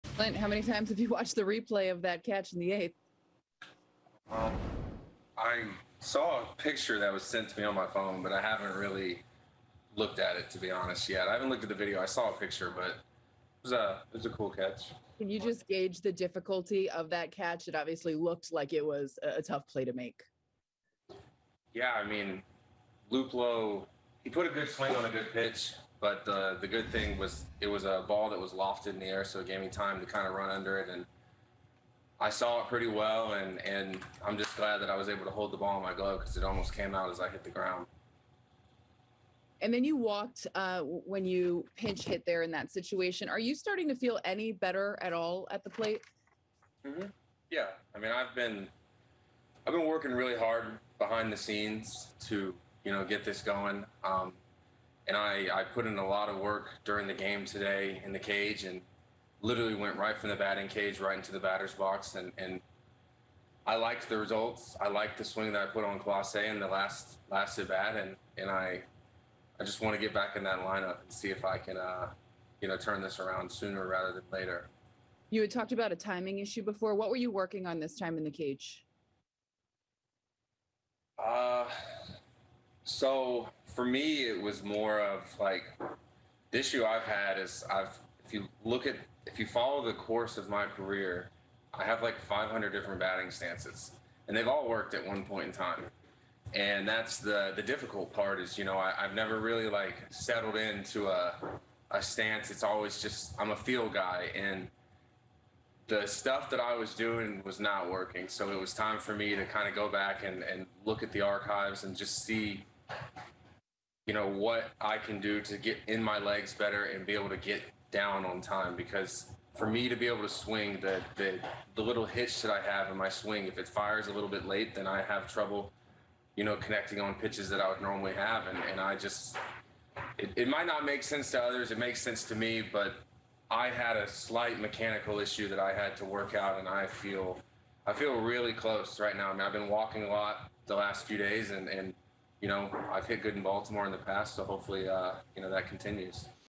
Clint Frazier Discusses His Highlight-Reel Catch
Clint Frazier shares his thoughts after a highlight-reel catch and a pinch-hit walk in the series finale in Cleveland.